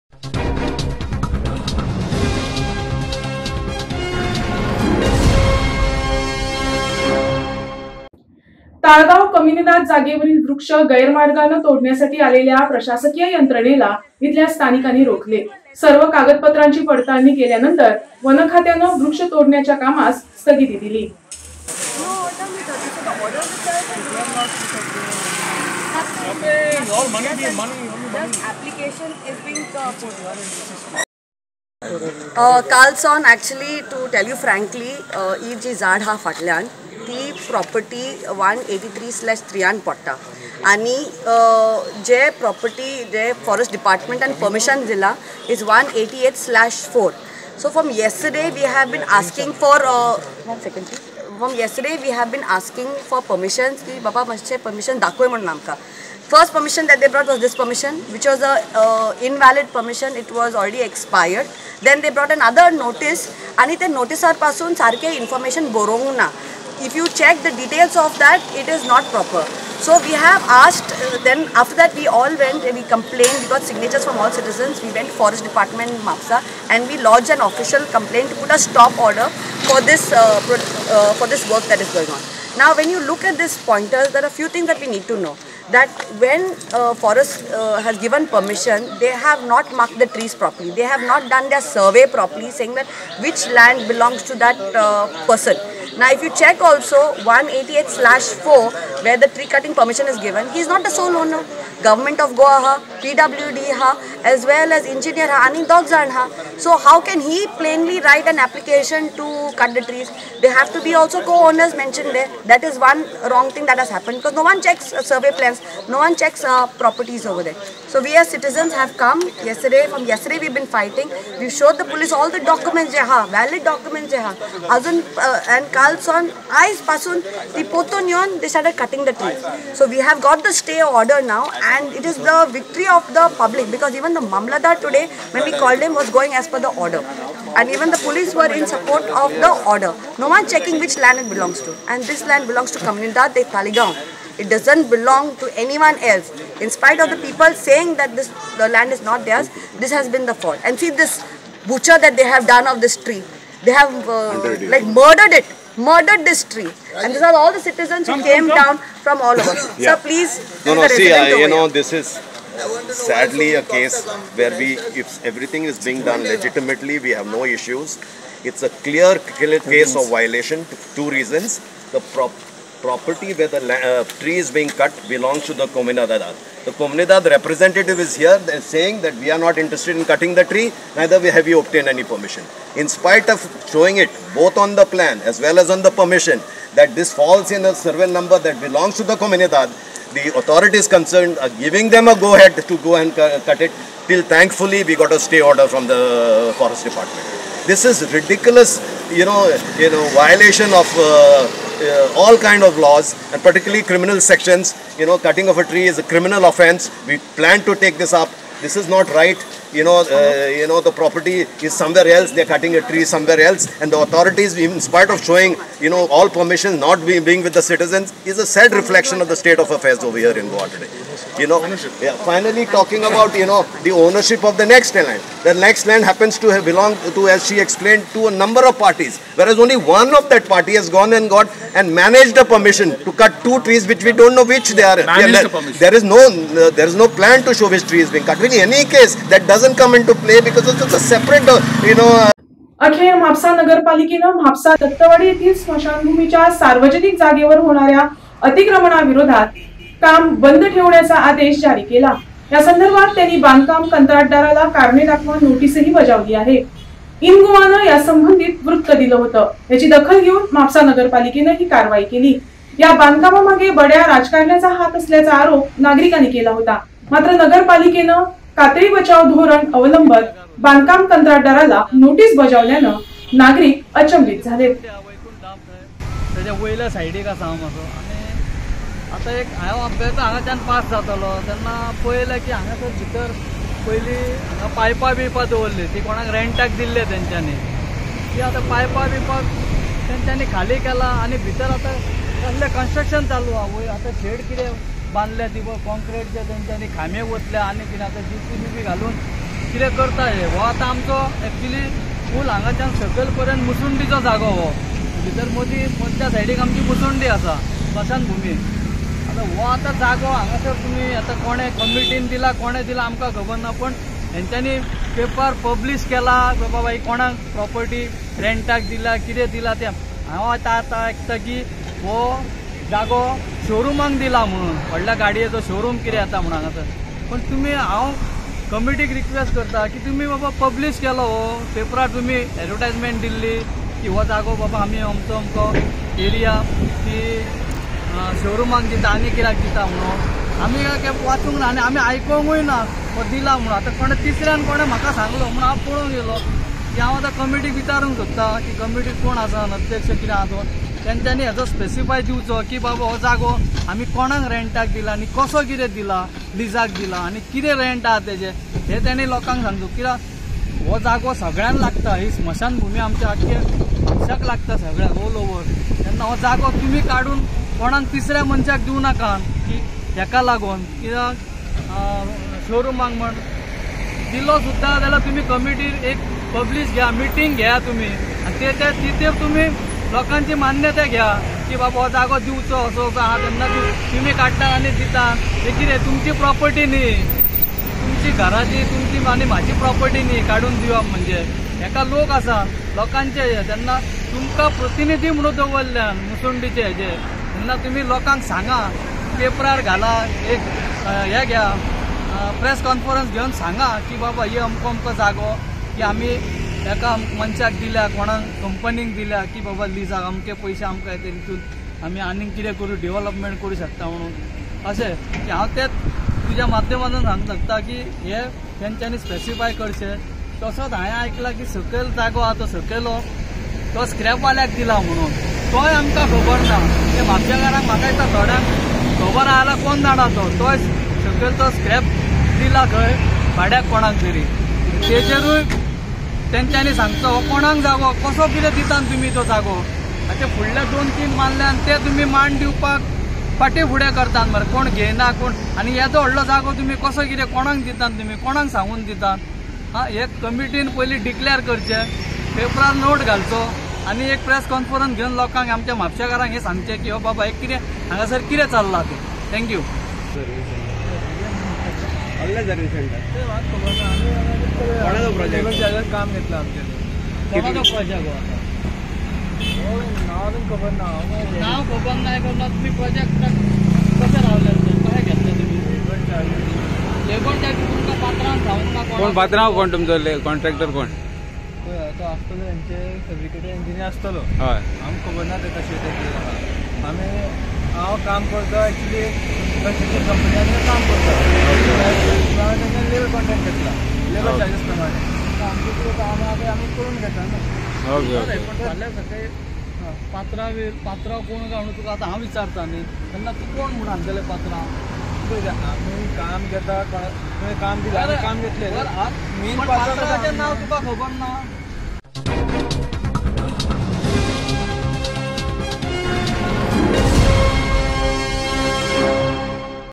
१. ताळगाव कोमुनिदाद जागेवरील वृक्ष २. म्हापसा स्मशानभूमी परिसरातील बांधकामास स्थगिती
१. ताळगाव कोमुनिदाद जागेवरील वृक्ष गैरमार्गाने कापण्यासाठी आलेल्या प्रशासकीय यंत्रणेला इथल्या स्थानिकांनी रोखले .सर्व कागदपत्रांची पडताळणी केल्यानंतर वन खात्याने वृक्ष कापण्याच्या कामास स्थगिती दिली

२. दत्तवाडी म्हापसा येथे सुरु असलेल्या बांधकाम प्रकल्पाचे काम बेकायदा असून सुरु असलेले काम त्वरित थांबिण्याचे आदेश म्हापसा पालिकेने बिल्डर ला दिले . म्हापसा  पालिका अधिकाऱ्यांनी नुकतेच बांधकाम ठिकाणी पाहणी केली म्हापसा पालिकेच्या कोणत्याही परवानगी शिवाय याठिकाणी बेकायदा काम सुरु असल्याचे पालिका अधिकाऱ्यांनी तपास अहवालात नमूद केले आहे